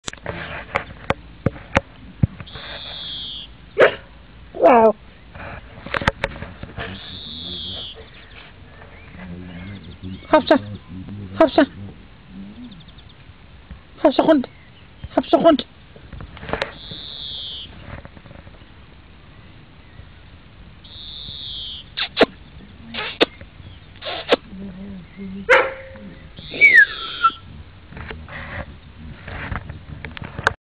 Samson - My Golden Retriever
My 40kg Golden Retriever named Samson - born and raised in the Kingdom of Denmark
when this was taken, he was approximately 2 years old
Before you rage about the lack of action in this movie. well.. i seriously do not care.. only uploaded to show a friend